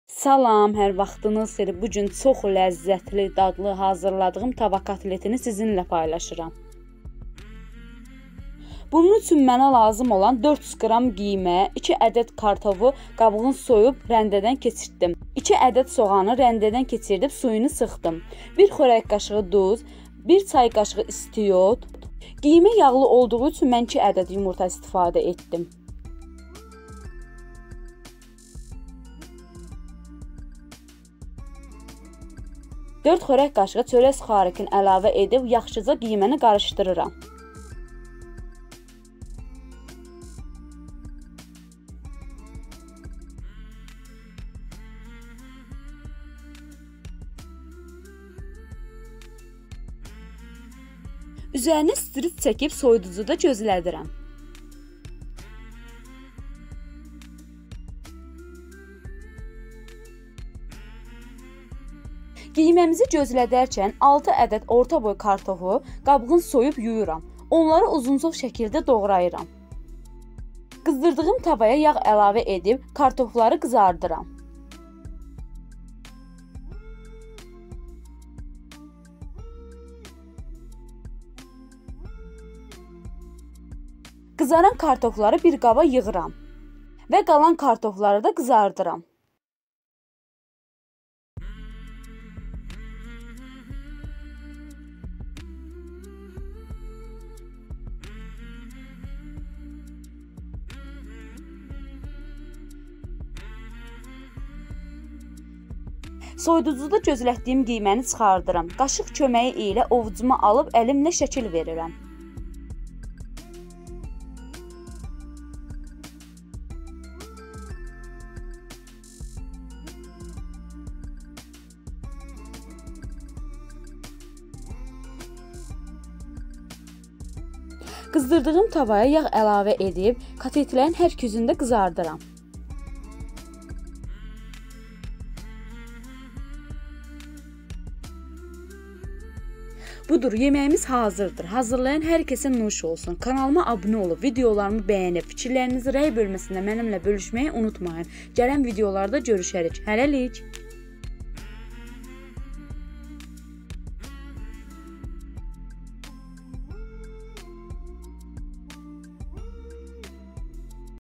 Tavada kotletin👌hazırlanması:)
400qiymə
2 ədəd kartofu
2 ədəd sogan
2 yumurta
4 xa suxarik çörək
duz-istiot
bisirmək ücun yağ